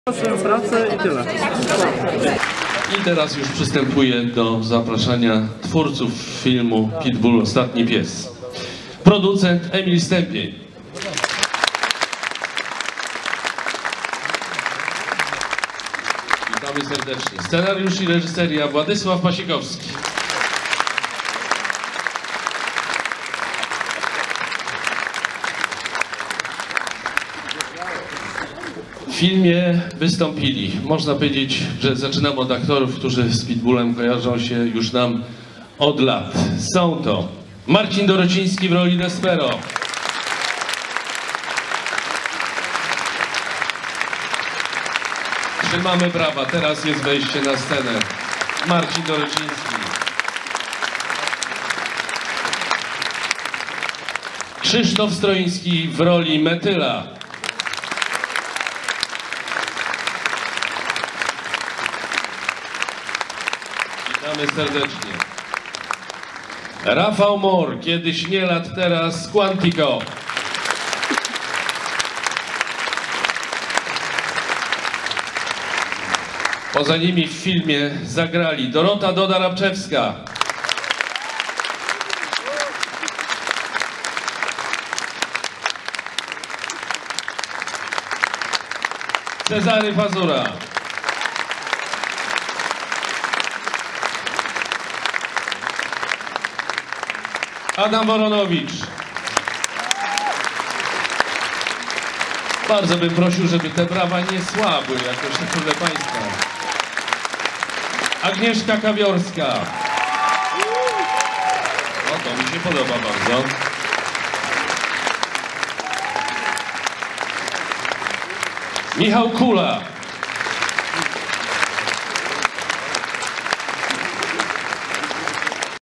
Pitbull.Ostatni pies. Migawka z premiery filmu 13.03.2018 Złote Tarasy, Multikino
Migawka z premiery filmu Pitbull.Ostatni pies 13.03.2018 Złote Tarasy, Multikino